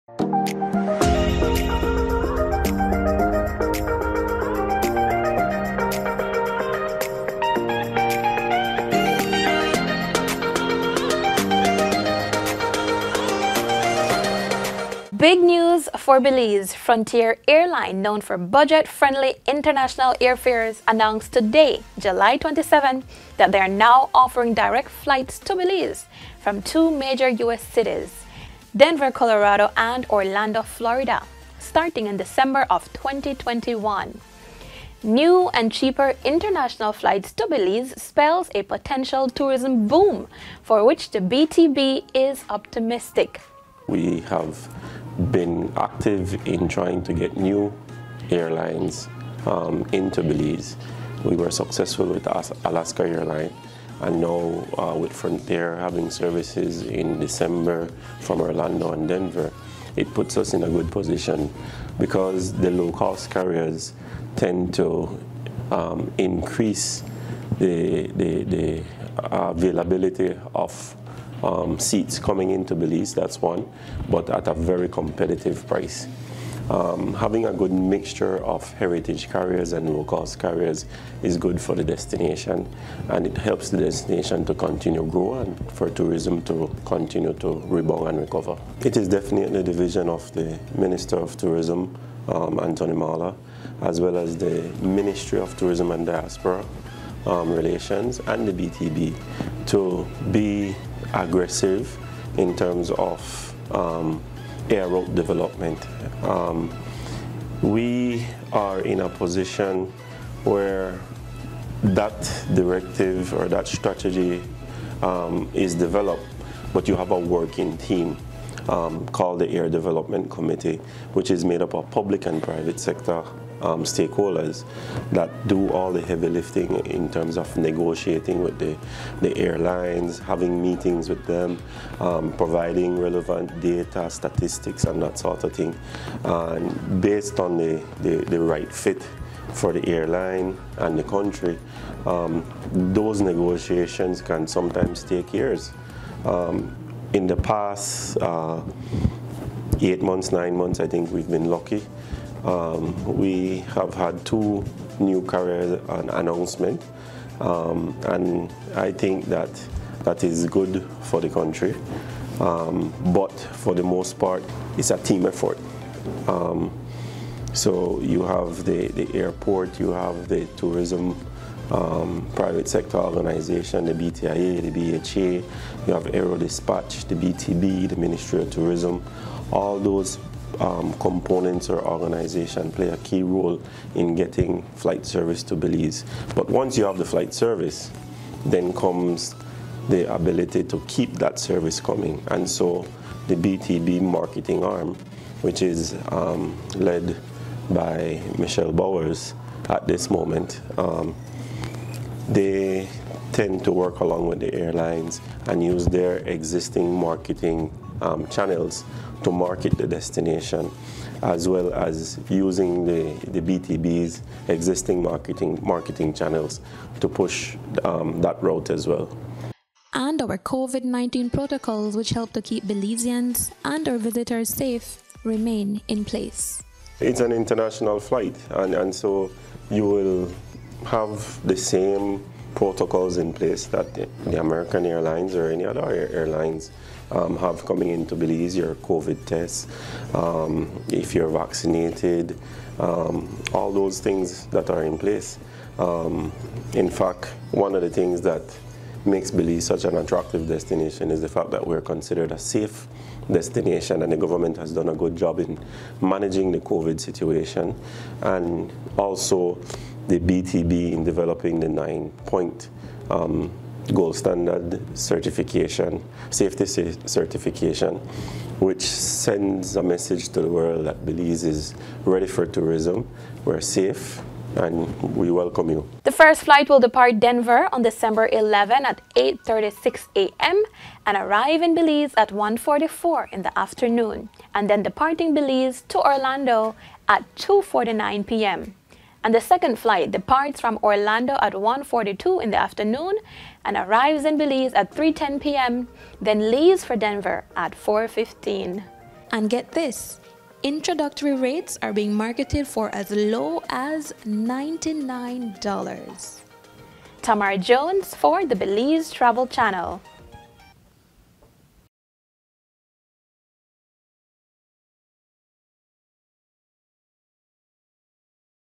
Belize Welcomes the Arrival of Frontier Airlines
Tonight, there is good news for the aviation industry as the Belize Tourism Board (BTB) has announced that come December eleven, Frontier Airlines will be providing a non-stop service to Belize from Denver, Colorado, and Orlando, Florida. We join our colleagues at the Belize Travel Channel for a look at this pending new arrival.